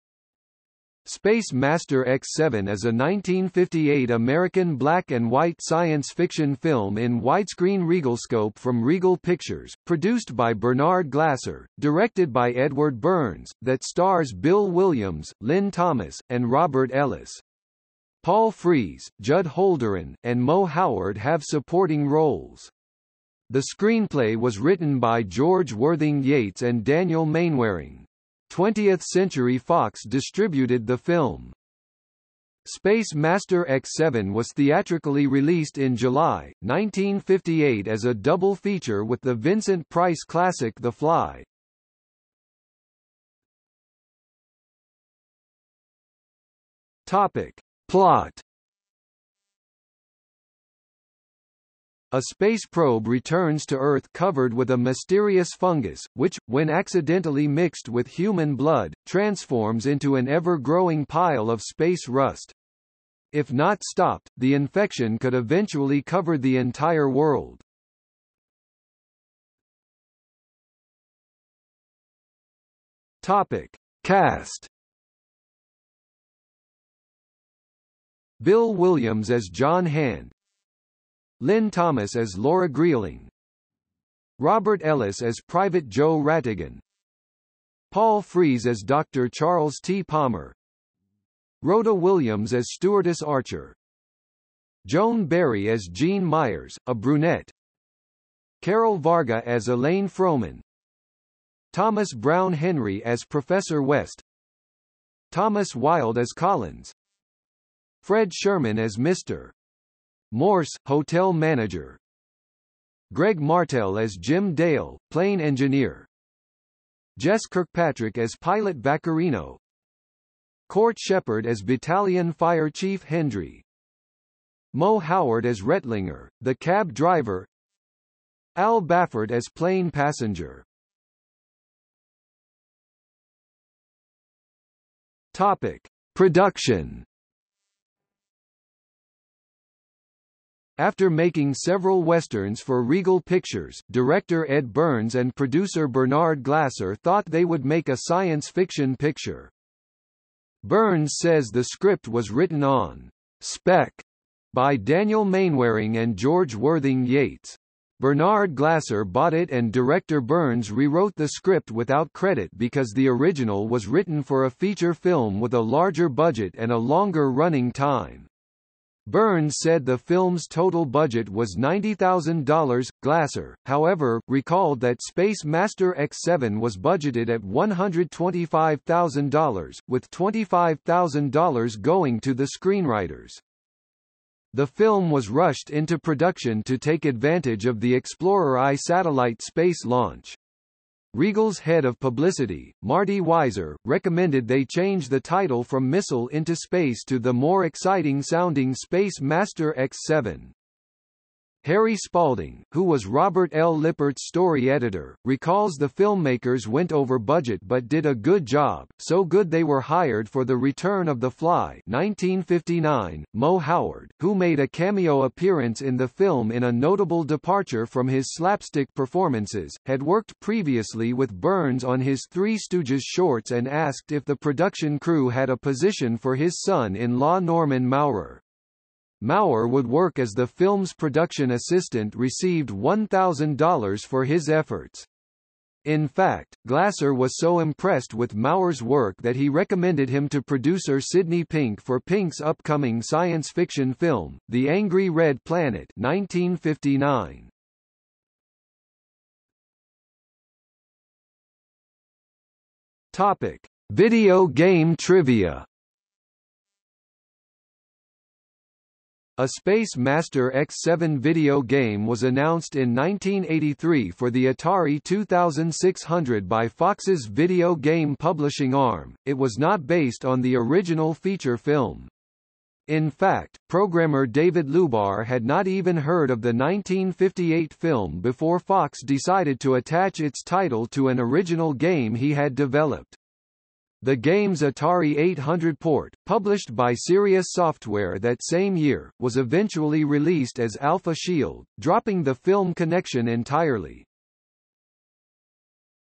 Space Master X-7 | Wikipedia audio article
00:00:43 1 Plot
00:01:09 2 Cast
00:02:11 3 Production
00:04:09 4 Video game trivia

- Socrates

SUMMARY
Space Master X-7 is a 1958 American black-and-white science fiction film in widescreen Regalscope from Regal Pictures, produced by Bernard Glasser, directed by Edward Bernds, that stars Bill Williams, Lyn Thomas, and Robert Ellis. Paul Frees, Judd Holdren, and Moe Howard have supporting roles. The screenplay was written by George Worthing Yates and Daniel Mainwaring. 20th Century Fox distributed the film.
Space Master X-7 was theatrically released in July, 1958 as a double feature with the Vincent Price classic The Fly.